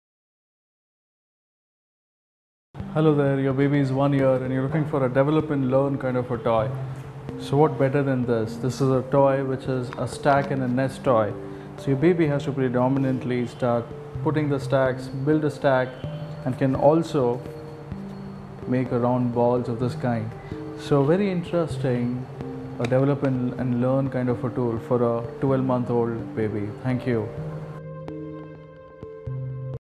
Stack 'n Nest 'n Roll for toddlers | Best toy for 1 year olds | Shopping Adviser
Stack 'n Nest 'n Roll for toddlers is a very good toy for developing children to promote their creativity. This toy can be stacked to form a ball. The varied colors also help in getting the kid familiarised with colors.
It is one of the best toys for 1 year old babies, which will help them learn and develop their cognitive skills.

For digital marketing services, to increase sales online or offline,